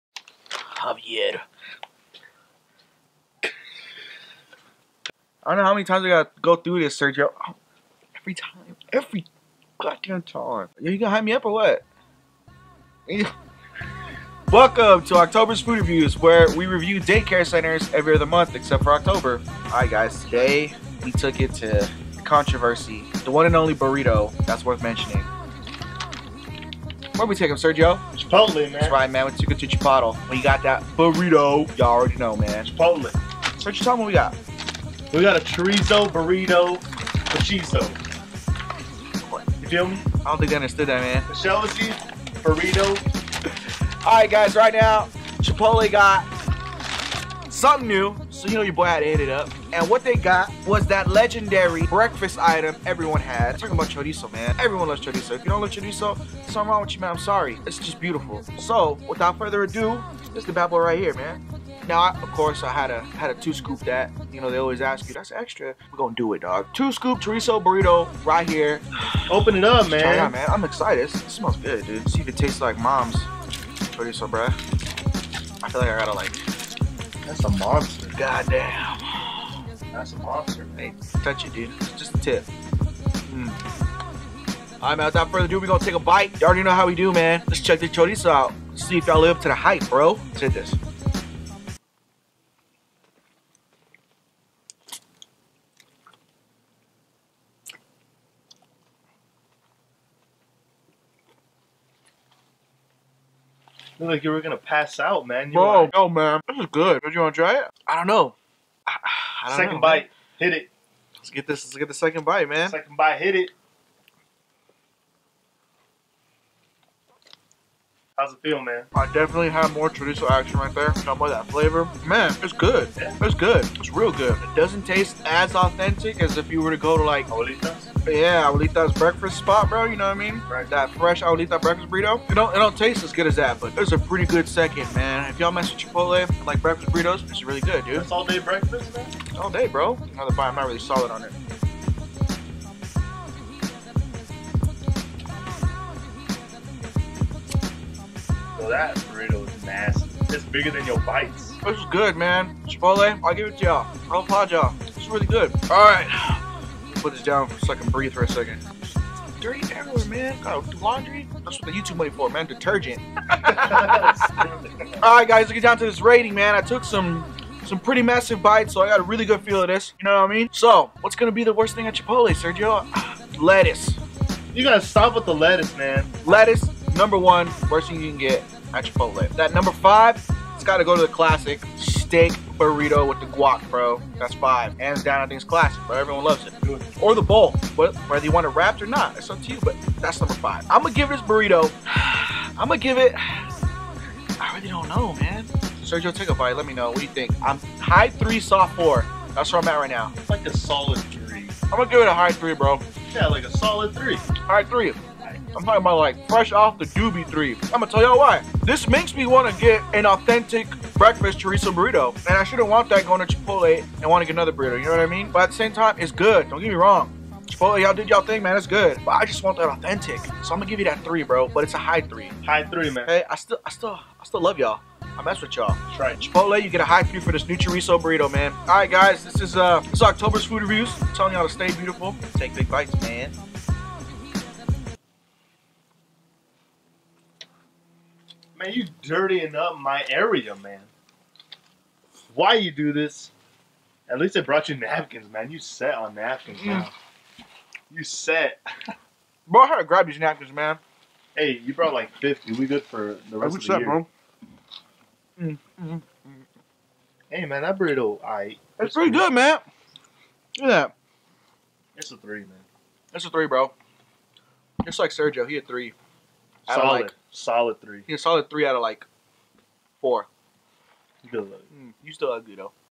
CHIPOTLE CHORIZO BURRITO!
We take a BIG bite into the Chipotle Chorizo Burrito!!